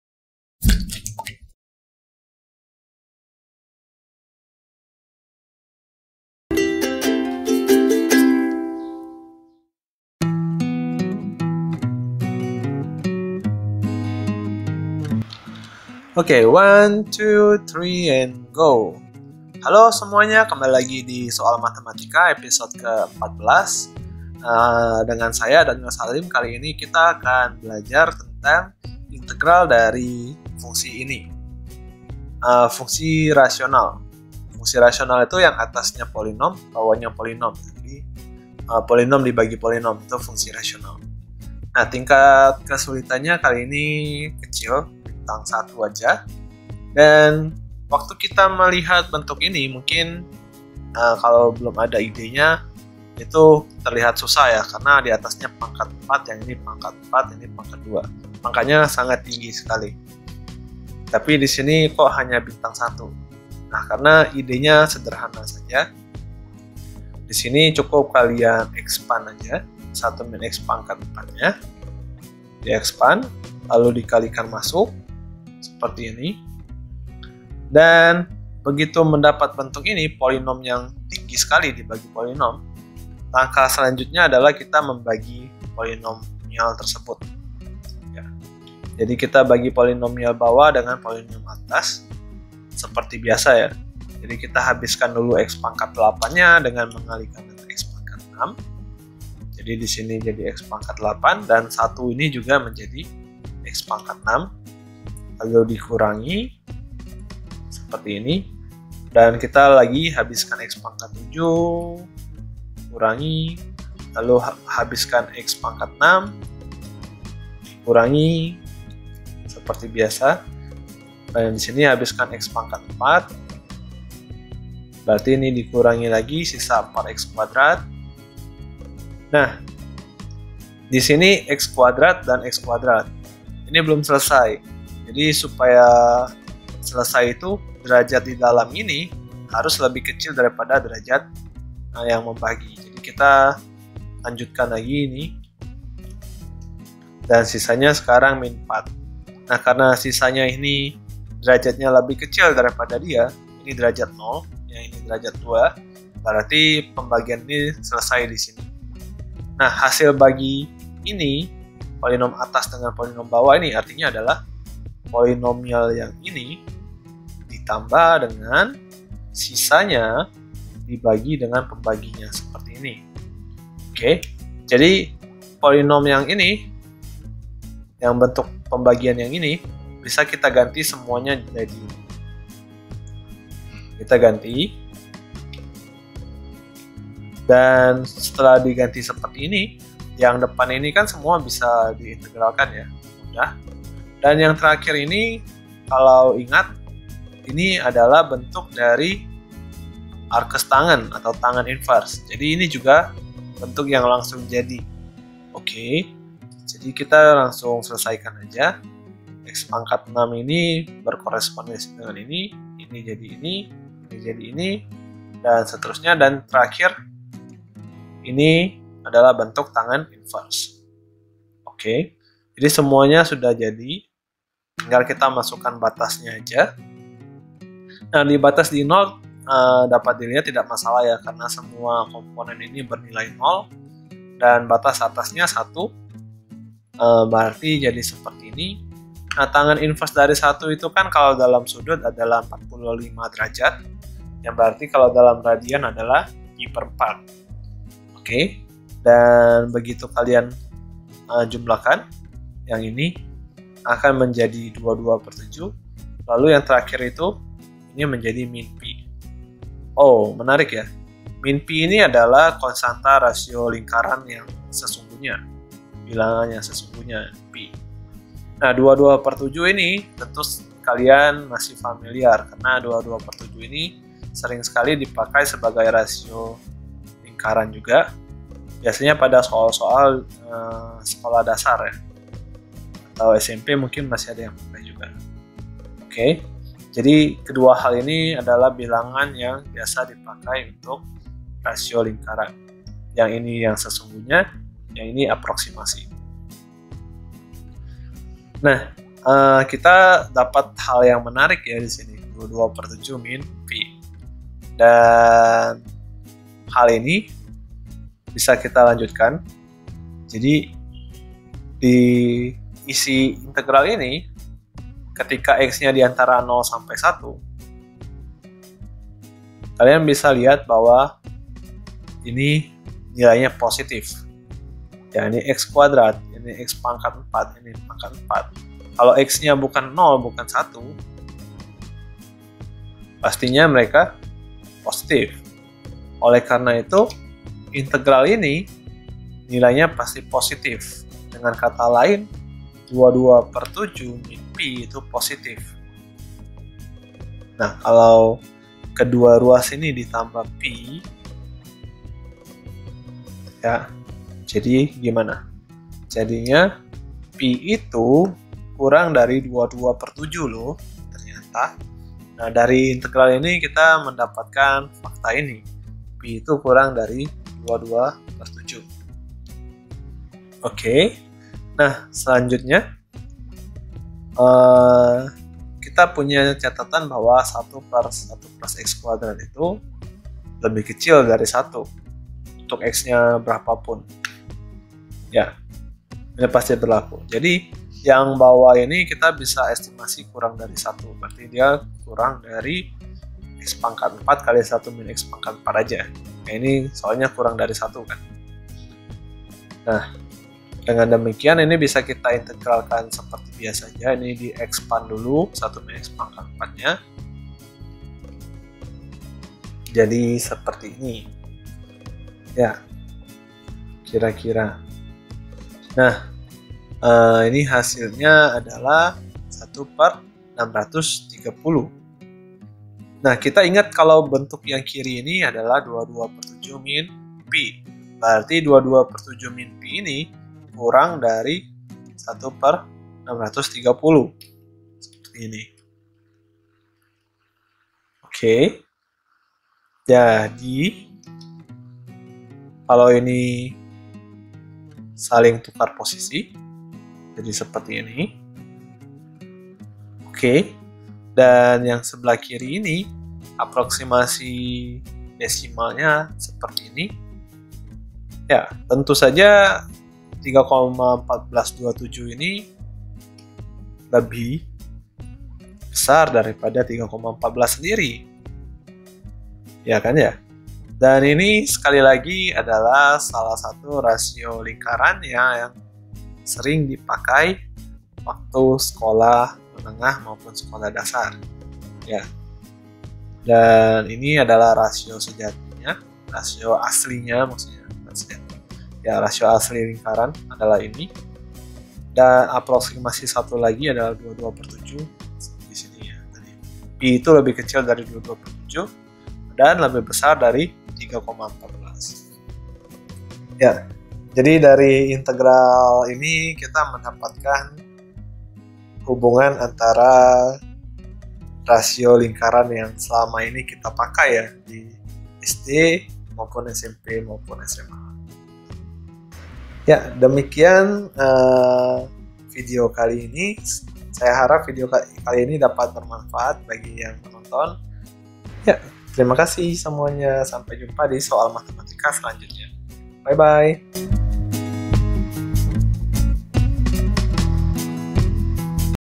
14-Bukti dengan integral
Konstanta lingkaran adalah pi~
Tapi di SD, kita sering menggunakan angka 22/7 atau 3,14 untuk
mengganti peran pi.

Lalu, bagaimana urutan 3,14 pi dan 22/7, siapa yang paling besar?
Siapa yang paling kecil?